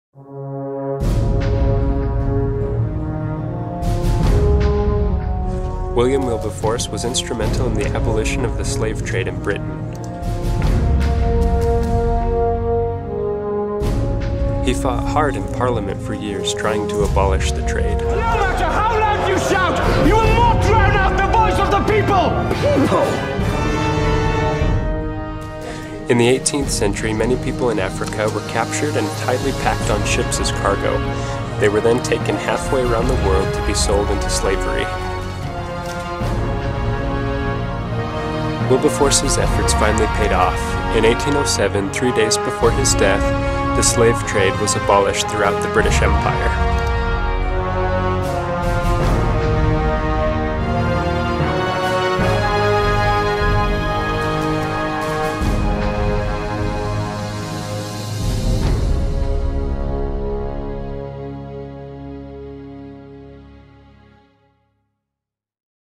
William Wilberforce: The Liberator
This is a brief overview of the impact that William Wilberforce had on the abolition of the Slave Trade in Britain during the late 18th and early 19th centuries.